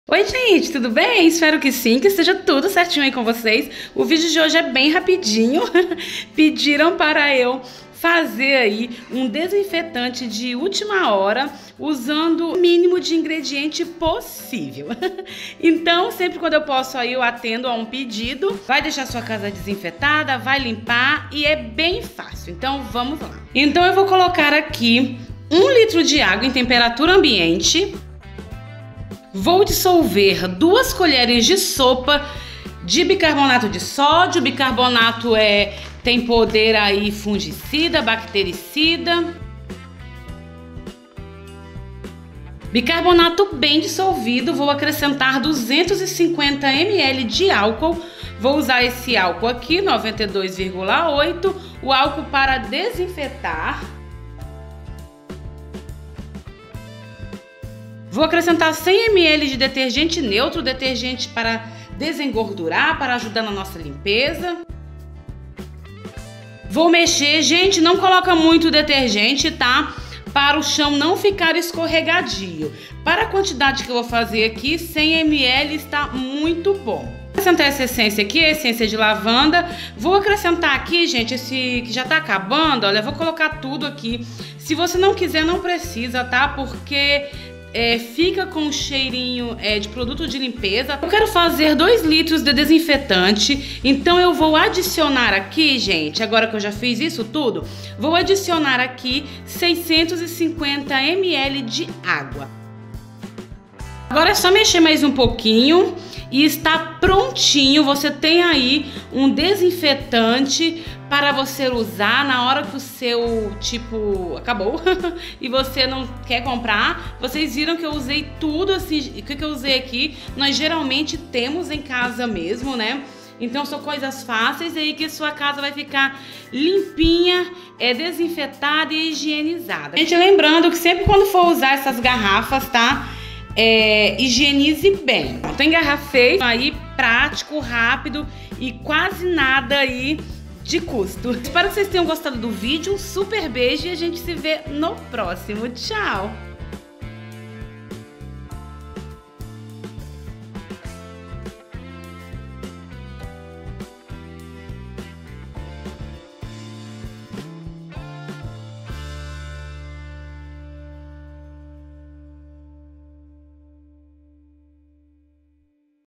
SEU DESINFETANTE ACABOU? FAÇA ESSE COM O QUE VC TEM EM CASA MUITO FÁCIL
* Ingredientes aqui embaixo e teste no final do vídeo*
ESPERO QUE GOSTEM E SE VOCÊ GOSTOU CLIQUE NO JOINHA PARA AJUDAR NOSSA COMUNIDADE.

Ingredientes:
 1 litro de água em temperatura ambiente;
2 colheres cheia de bicarbonato de sódio;
250 ml de álcool;
100 ml de detergente;
essência (opcional);
650 ml de água em temperatura ambiente.

SUPER BEIJO NO CORAÇÃO E ATÉ O PRÓXIMO VÍDEO.